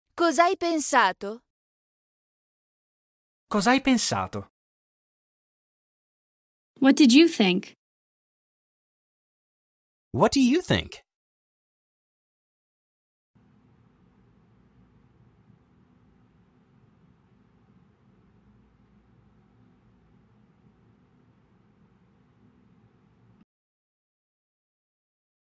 How to say "What did you think?" in Italian
Male Voice:
Cos'hai pensato?

Female Voice:
Cos'hai pensato?